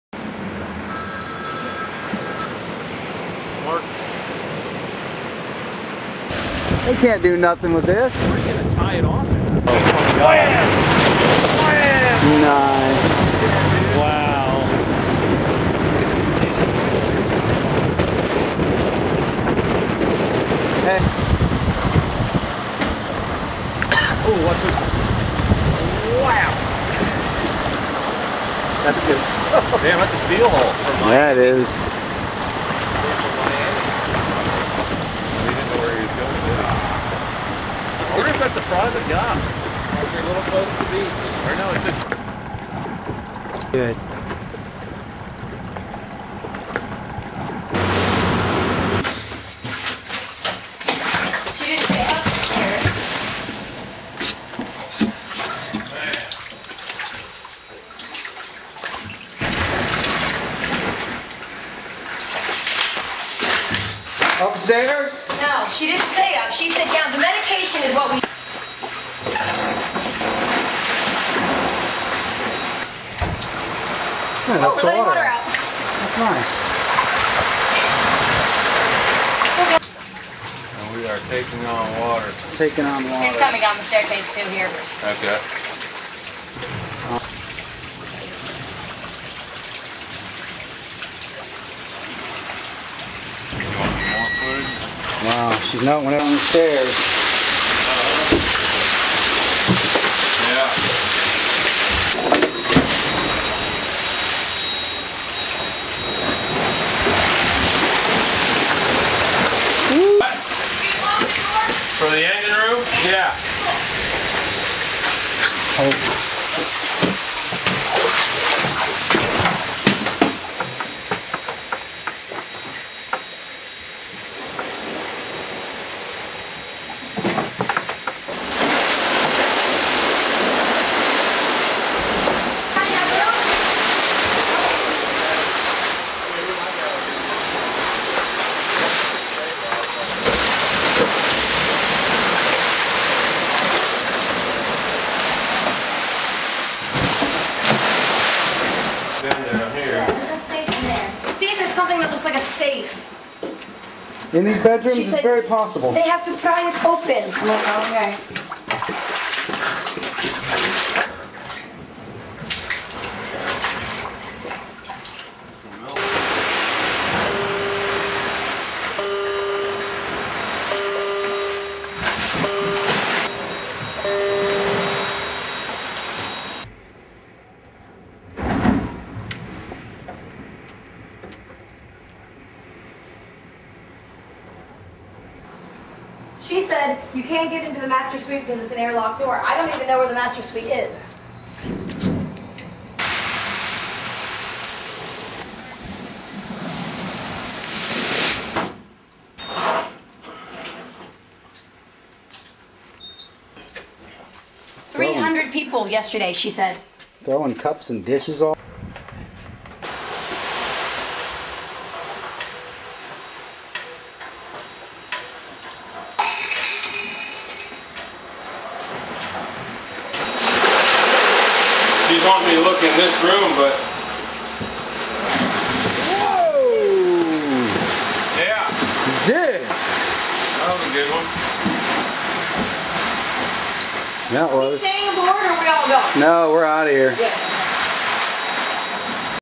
SINKING SHIP Terrible Disaster
Large Ship Aground Dashed by Large Waves. The Cruise ship Sir Winston ran aground off Longboat pass. Video courtesy of ezfishfinder.com